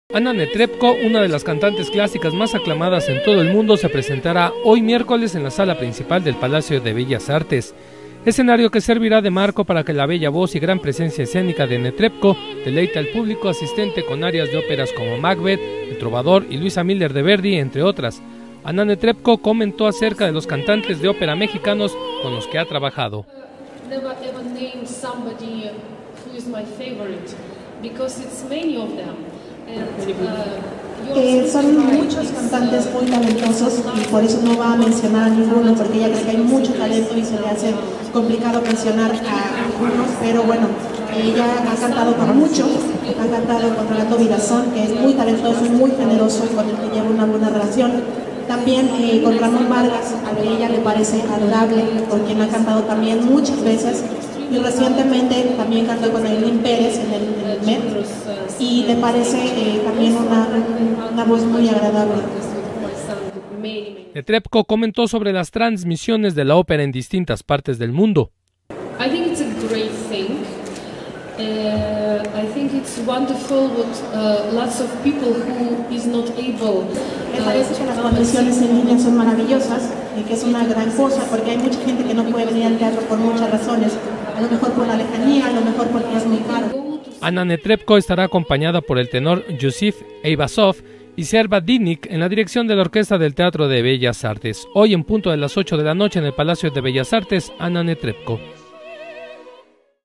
ANNA NETREBKO EN BELLAS ARTES
La cantante de ópera Anna Netrebko se presentará este miércoles 25 de febrero en la sala principal del Palacio de Bellas Artes a las 20:00 horas